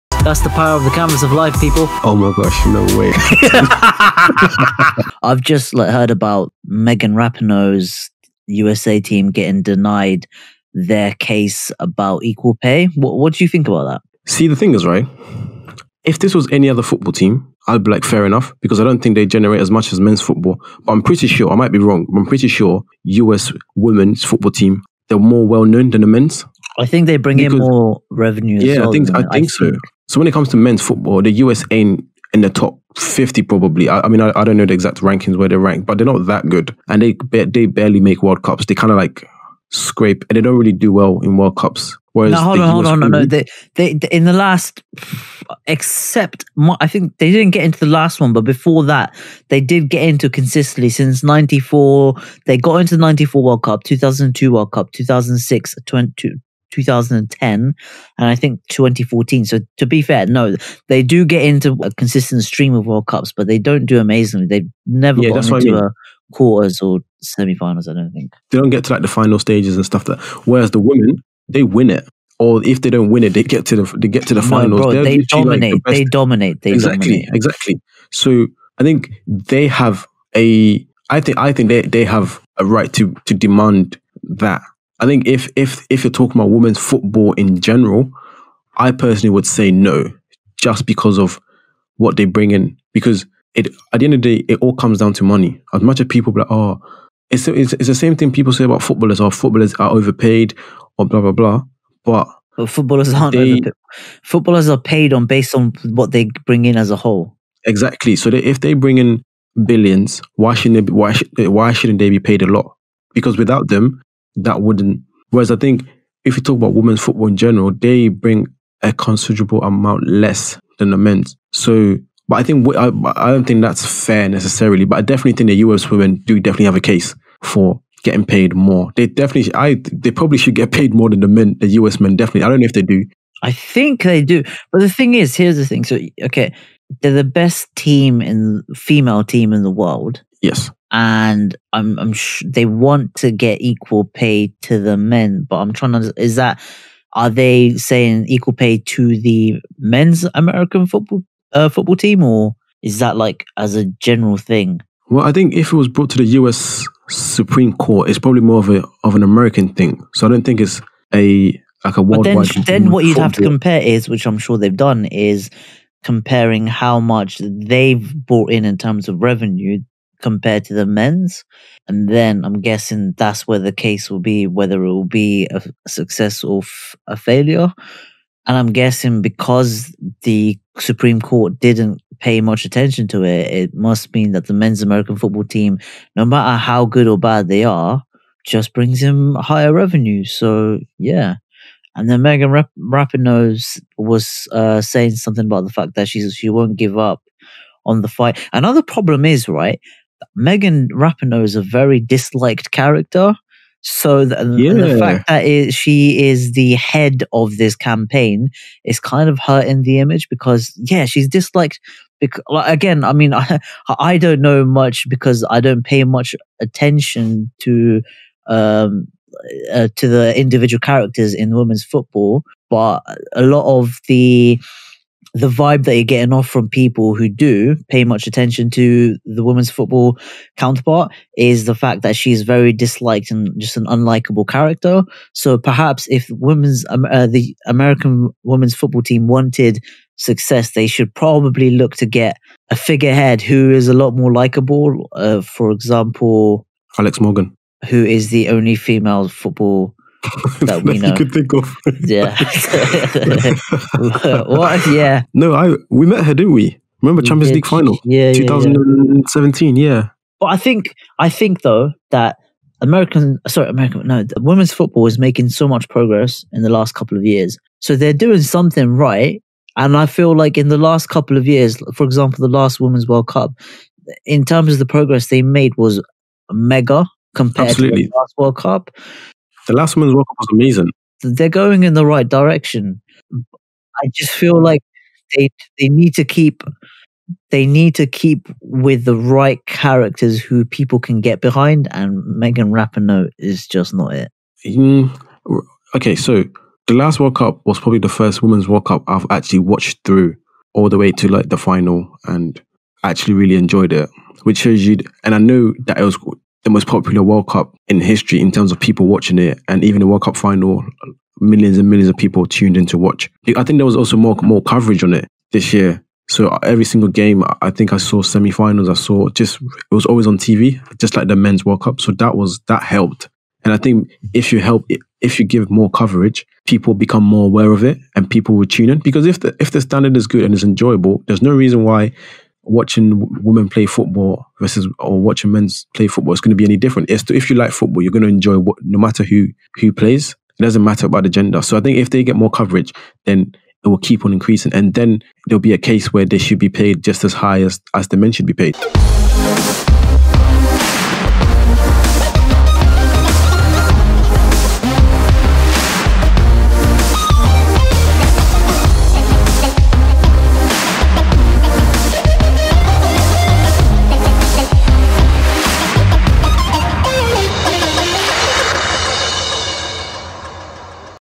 Should Female Footballers be Paid the Same as Men?
Should female footballers be paid the same as men? As viewing figures and attendance records for women's sporting events rocket, this is a question that needs exploring. Check out the video to find out our thoughts.